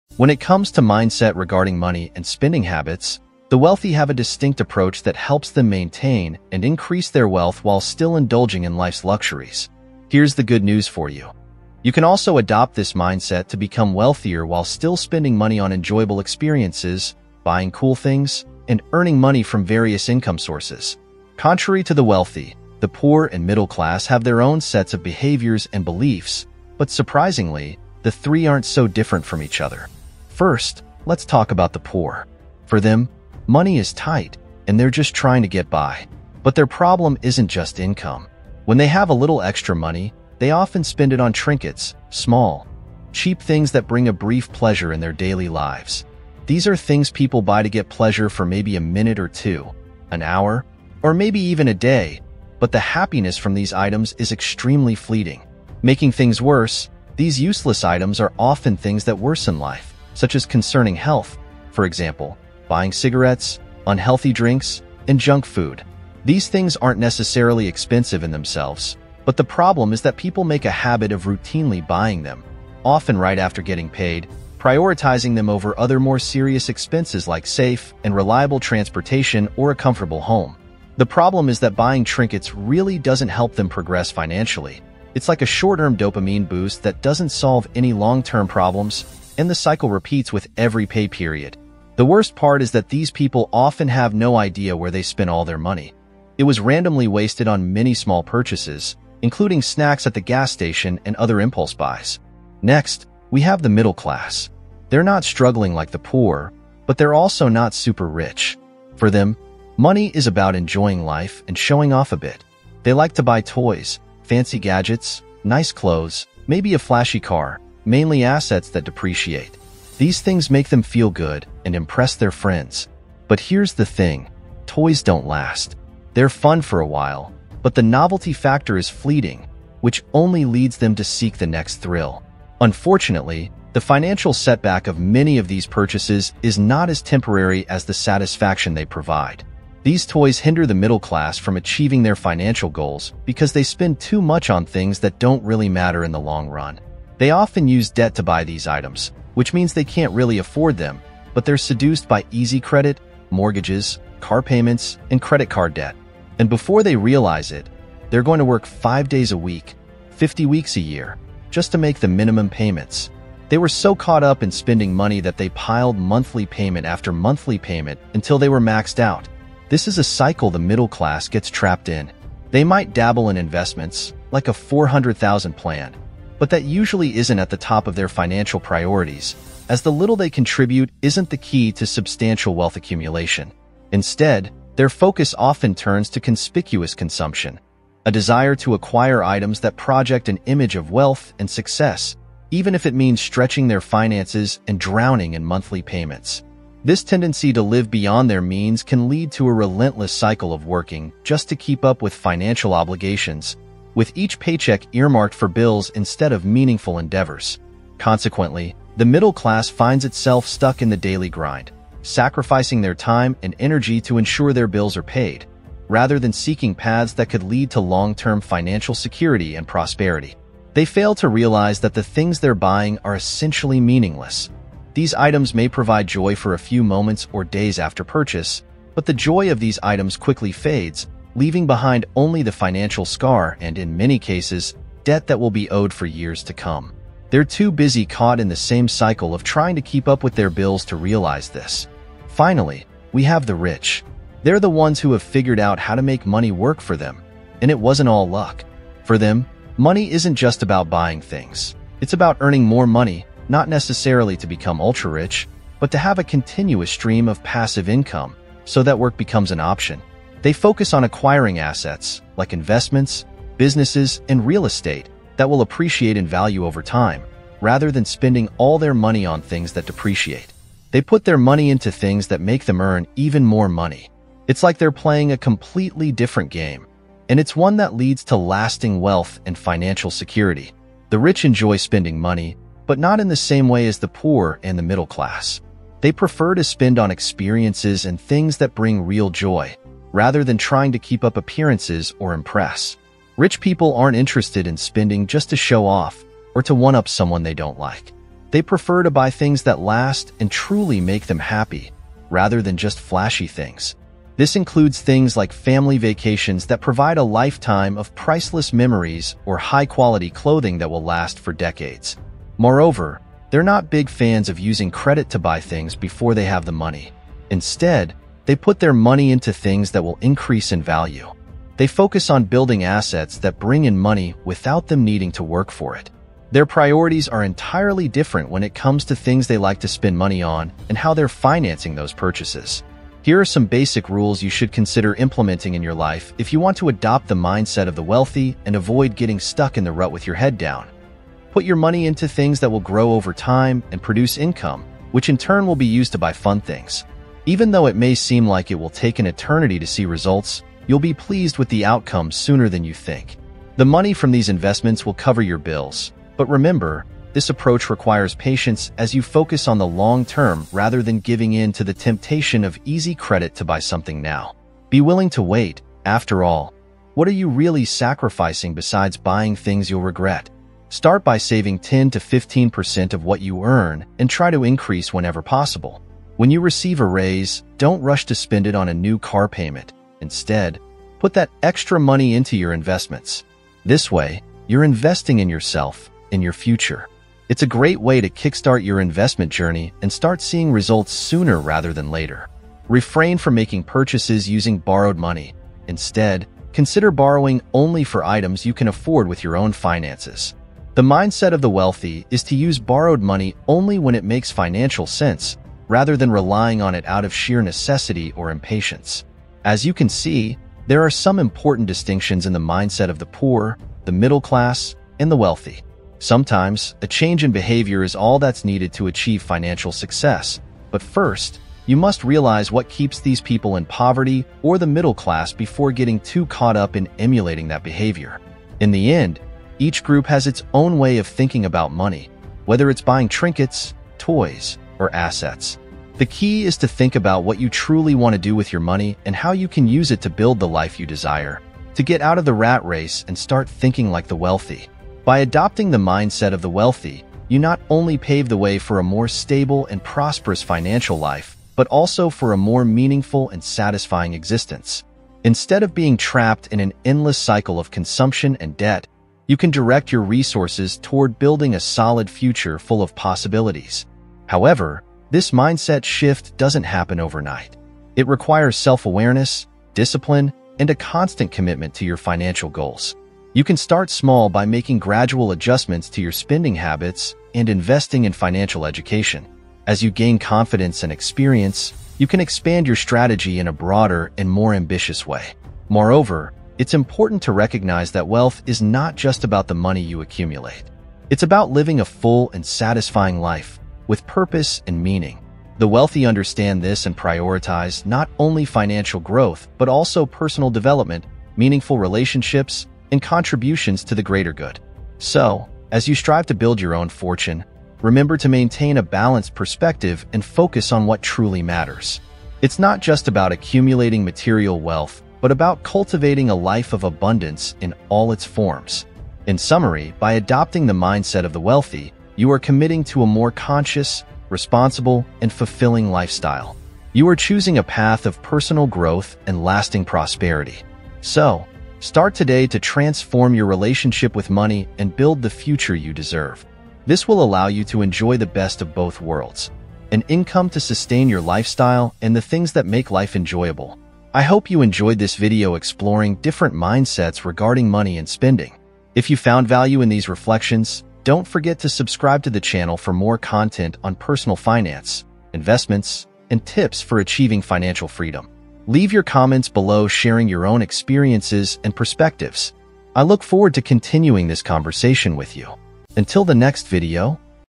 How the Rich Think, The Mental Map to Financial Success!
The video explores the different mindsets regarding money and spending habits among the poor, the middle class, and the rich. It highlights how the poor tend to spend on short-term items, while the middle class seeks status through purchases, often resorting to debt. On the other hand, the rich invest in assets that generate passive income, allowing them to enjoy life without relying on conventional work. The video offers practical advice for adopting a more effective financial mindset, such as prioritizing long-term investments and avoiding unnecessary debts.